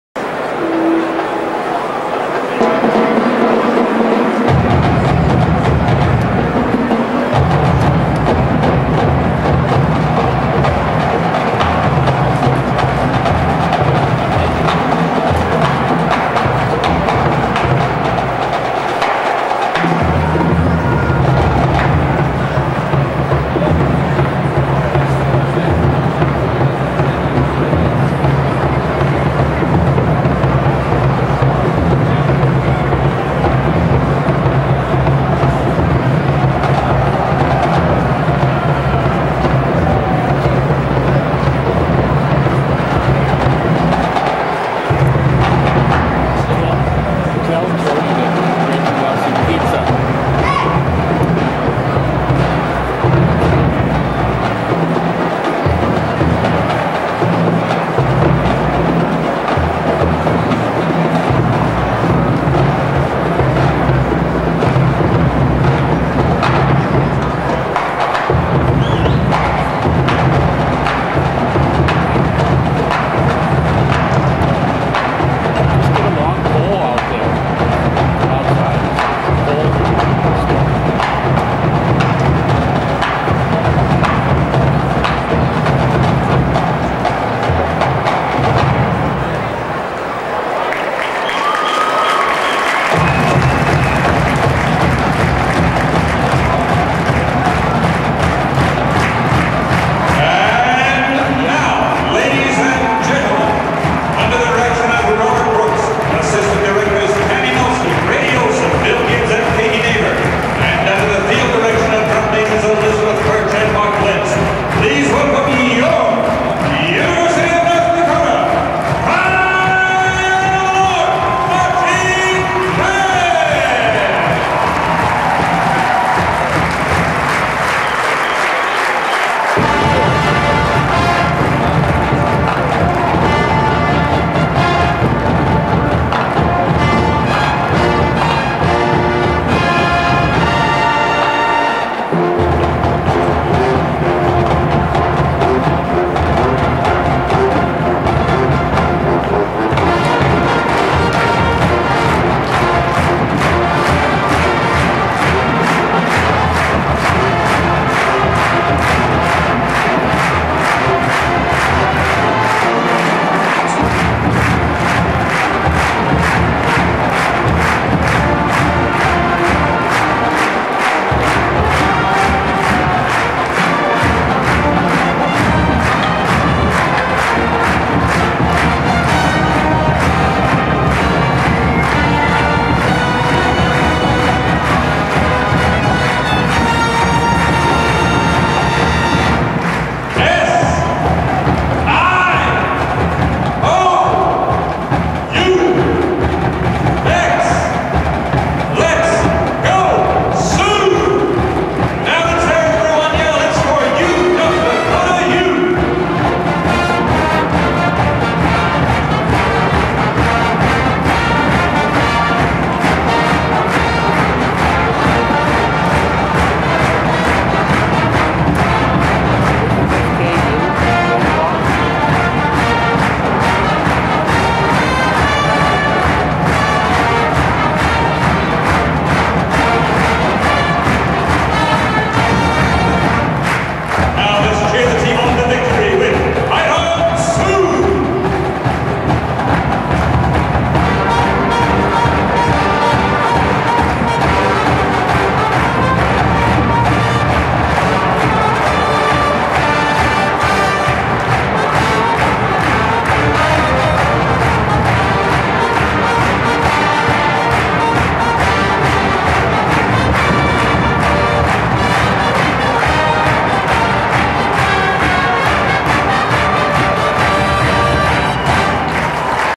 2003 UND vs. North Alabama Pride Pre-Game Show
The University of North Dakota Pride of the North Marching Band performs the fight songs.